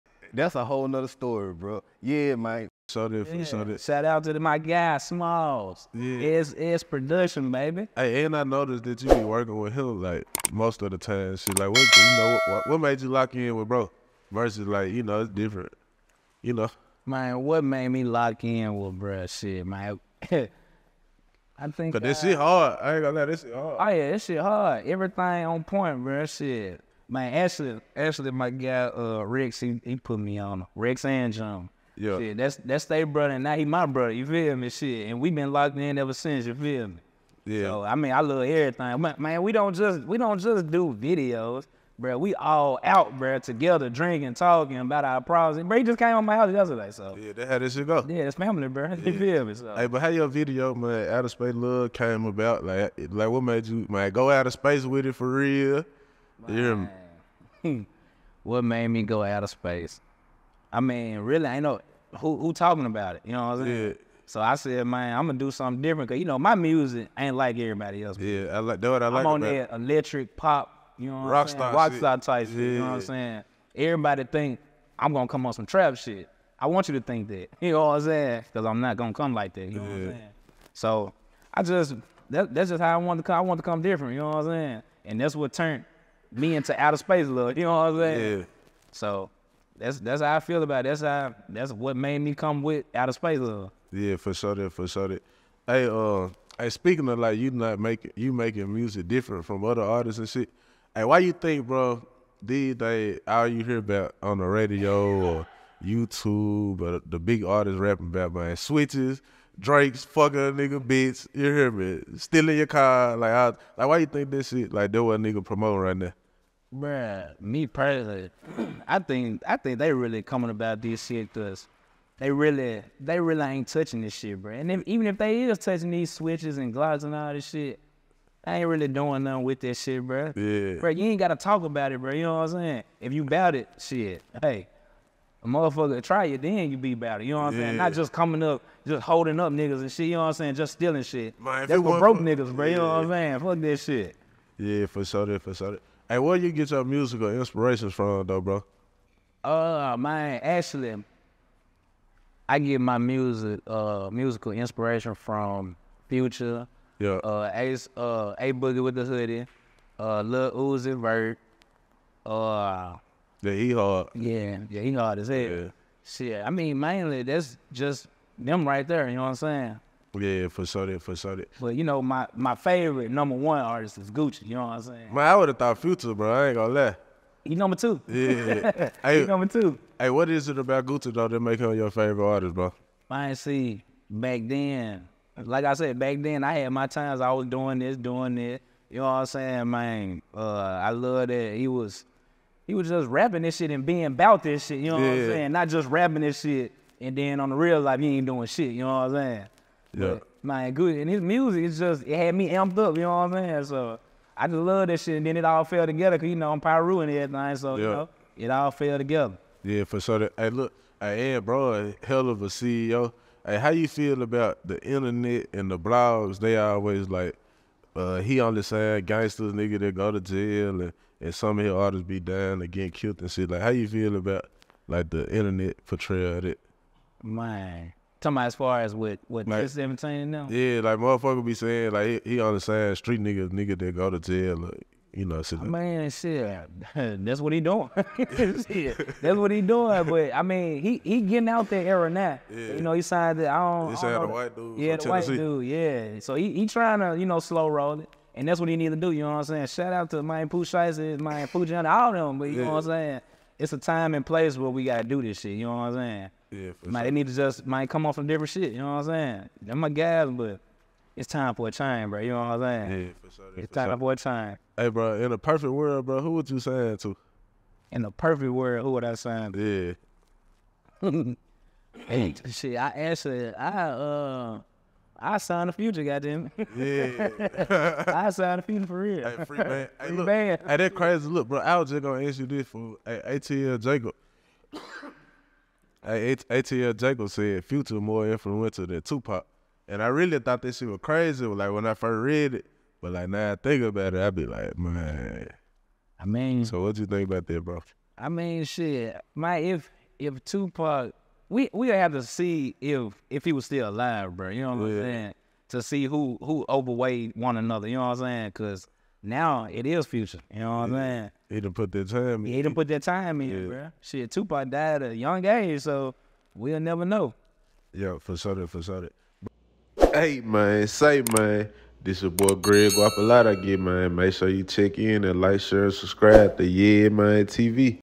Dirty Sprite on relationship with @SSproductions901 , Music Inspirations, Being PIRU on Yeamane TV
YeaManeTV: @YeaManeTV
Interviewer IG: @Greg_Bam
Filmed on location: Exit 221 Creative Studio